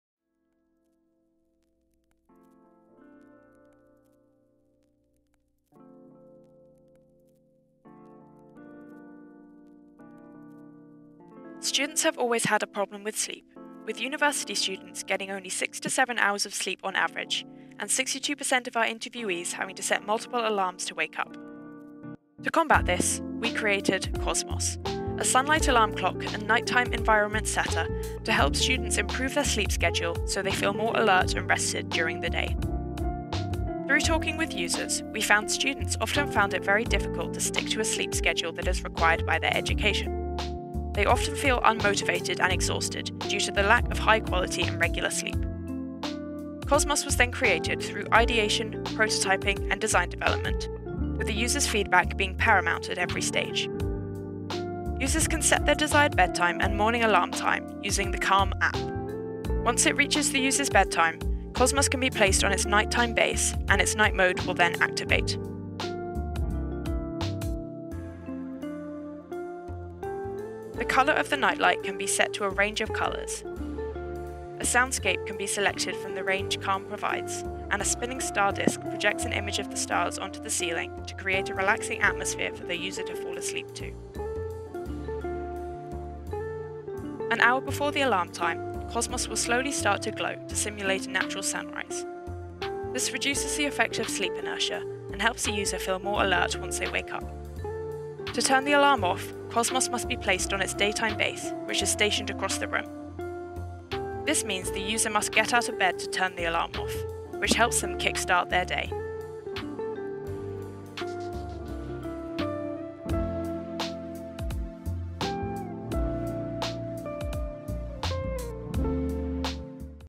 Team 01: Cosmos - DE2 - Industrial Design Engineering 2021
Students were asked to design and engineer a battery-powered hand tool for home, garden or educational use. The hand tool had to appeal to the mass market and also meet the needs of a specific (underserved) user group. The mission was to design and develop an inclusive consumer product that genuinely deserves to exist!